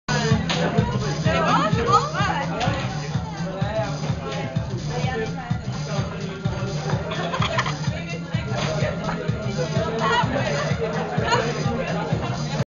Cul sec!!! (no.1)
Mélanie and Allyson drinking Becherovka